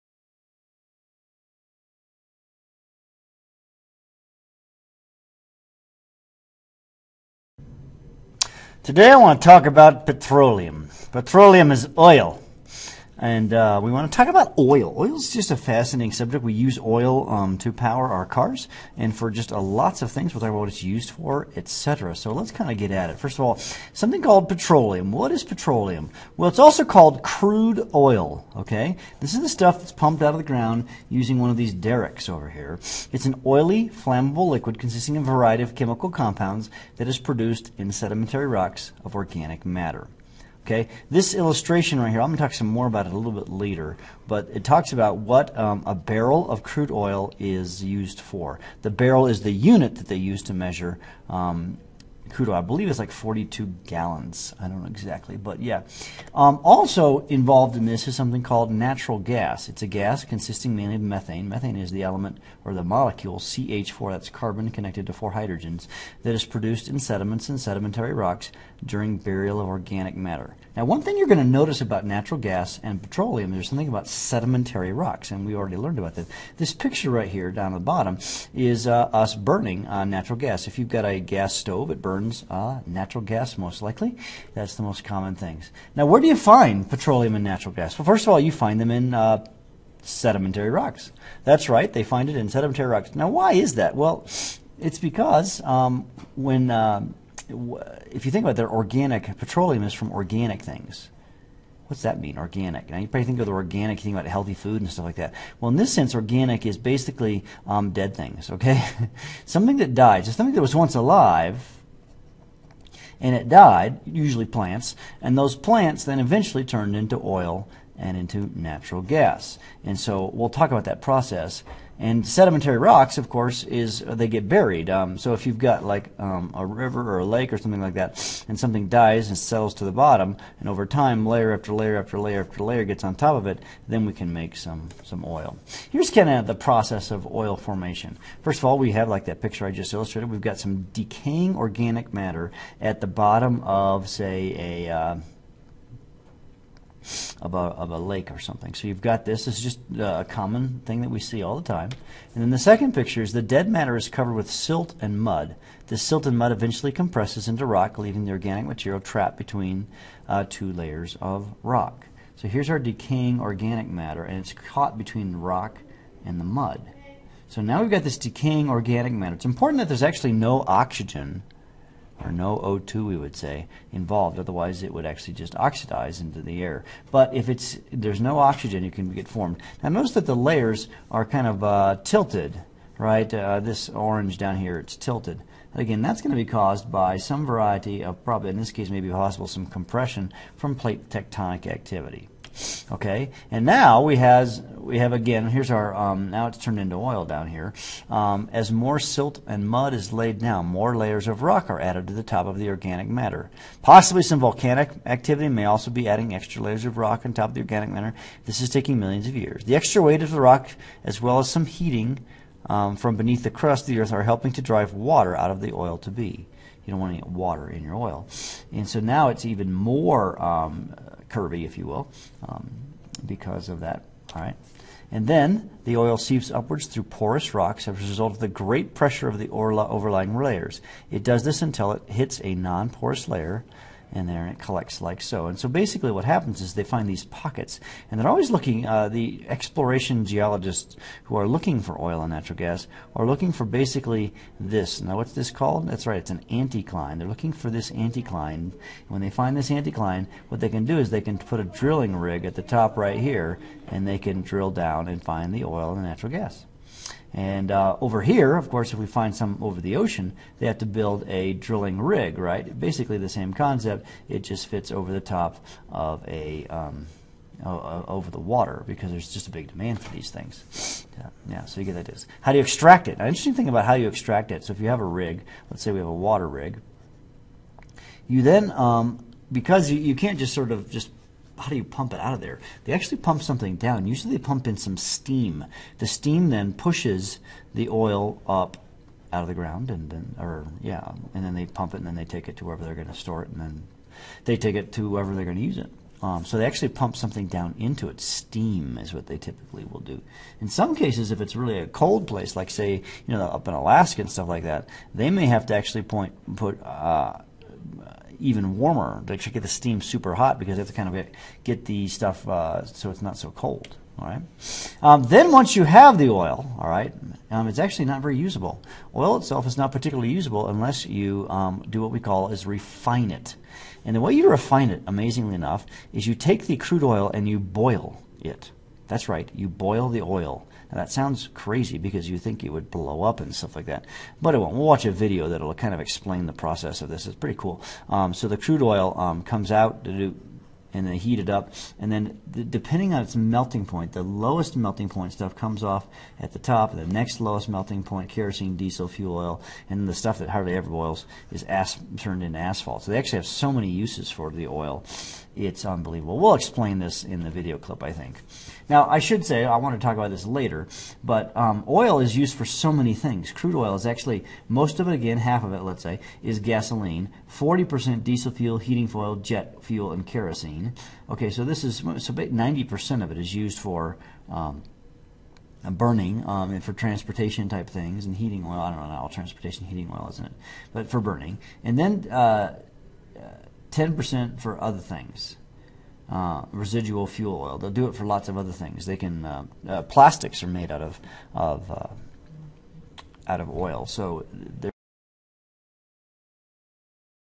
Where does Petroleum Come From and How do we Extract it? (1/3)
Join award winning teacher Jonathan Bergmann as he discussed petroleum.  Where does oil come from?  How do geologists decide where to drill it?  How is it separated?  .This video was made for his 9th grade students.